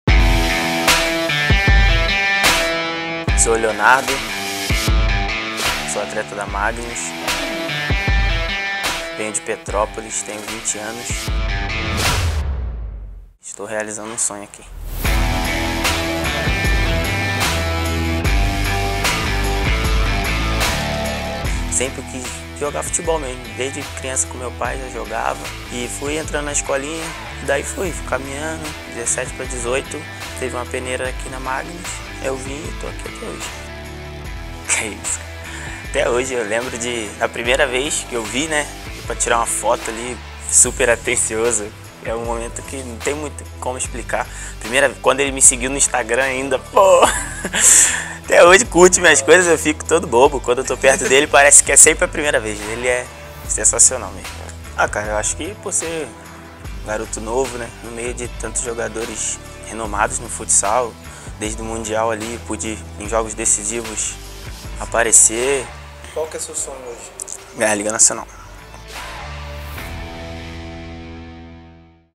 Leozinho, Melhor Jogador Jovem do Mundo em 2019
O ala do Magnus foi eleito, pela Futsal Planet, o Melhor Jogador Jovem do Mundo em 2019